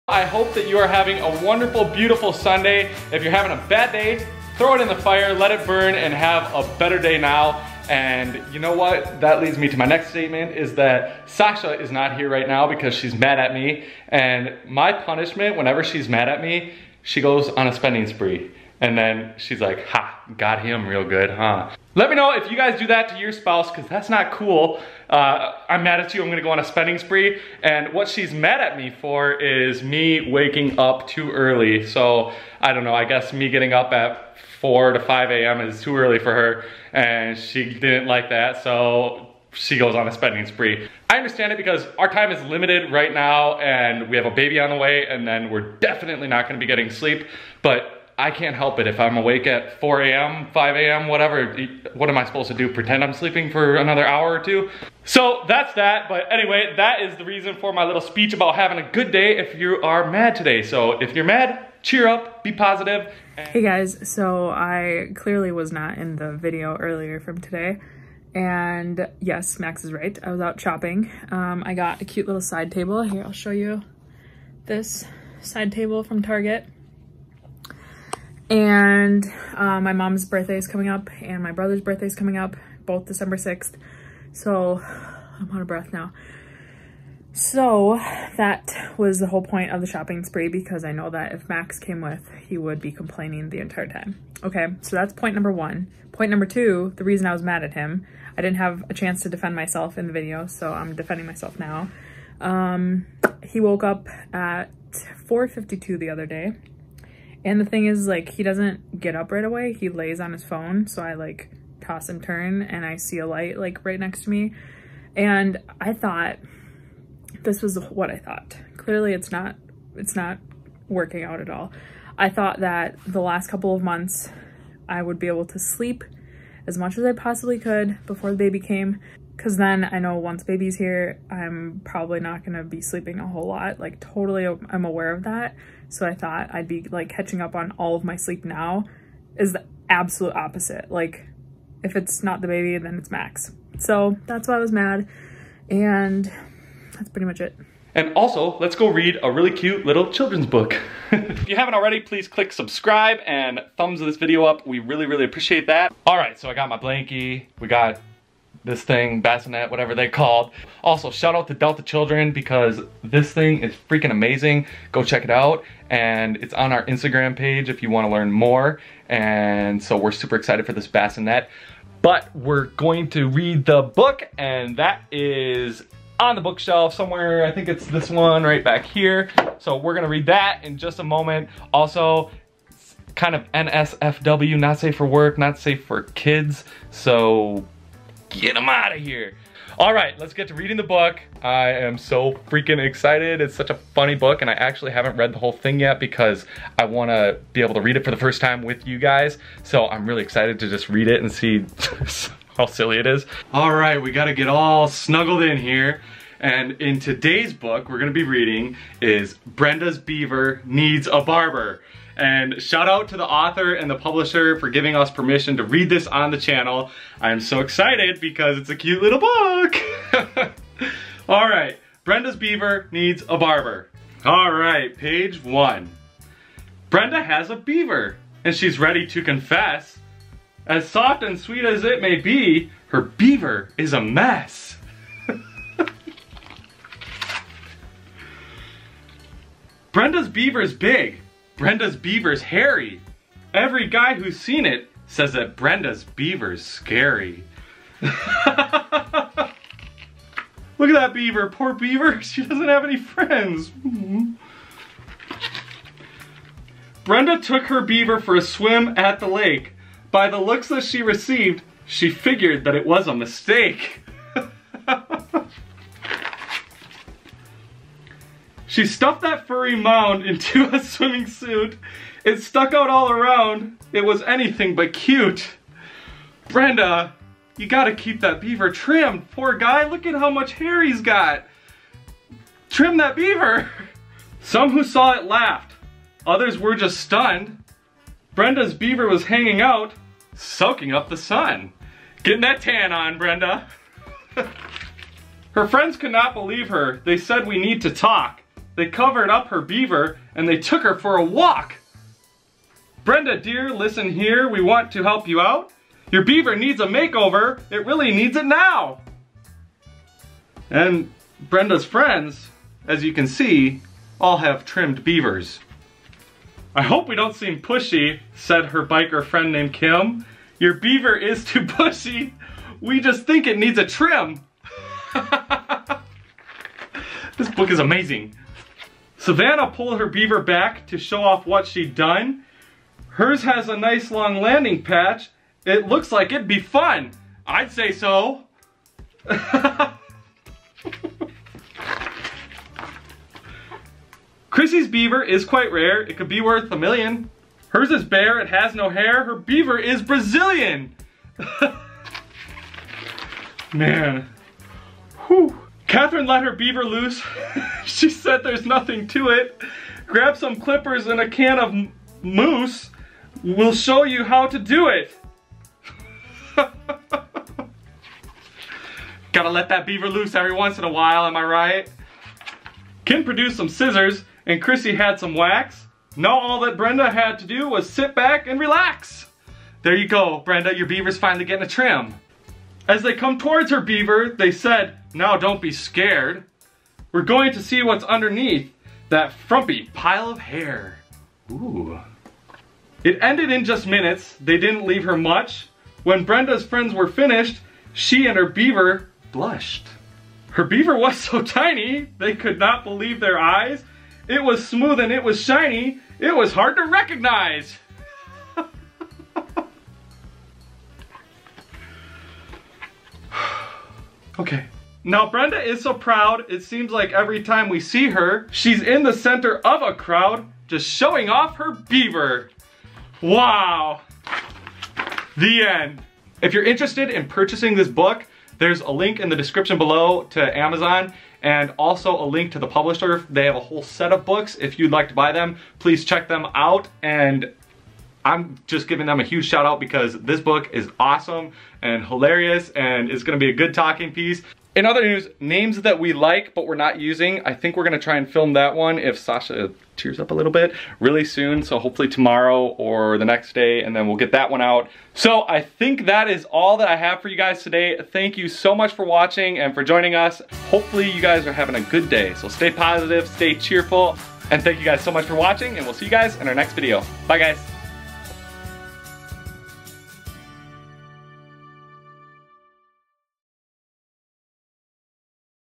*HILARIOUS* "kids" book - Brenda's Beaver Needs a Barber
Hey guys, Max did a little "storytime" reading for you today with the hilarious book "Brenda's Beaver Needs a Barber" by by Bimisi Tayanita (Author), Sumguyen Bangladesh (Illustrator). These are a great gag gift and great conversation piece, especially for the holidays!

We received full express written permission from the Author and Publisher to read this book on the channel.

5179 Main St E
Maple Plain, MN
55359